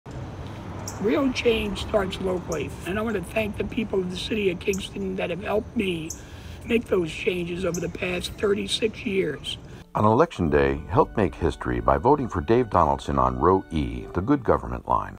Let's Make History Together - Vote Donaldson on Good Government line Row E
David Donaldson works for the betterment of Kingston and Ulster County and will continue to make positive changes for our community.

Learn more about Dave and his history on DavidBDonaldson6.com